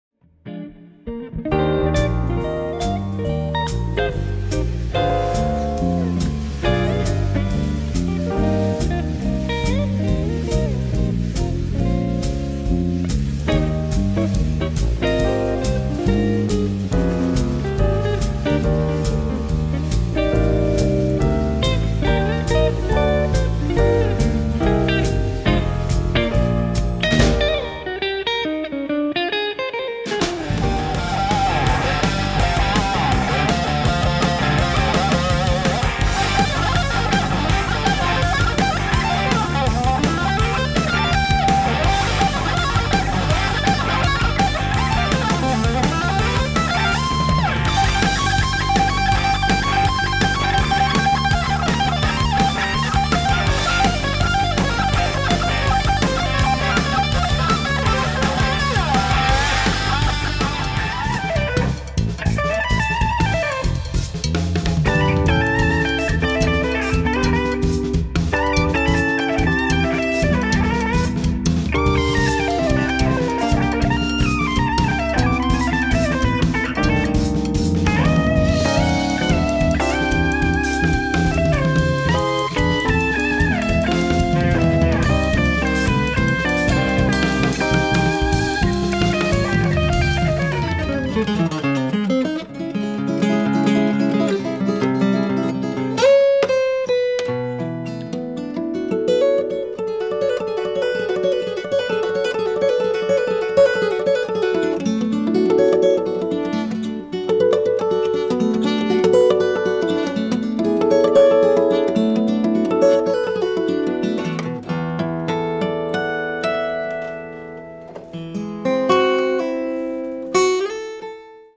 Hunter Bishop Senior Recital Highlights
Live performance highlights from a Belmont University Senior Recital.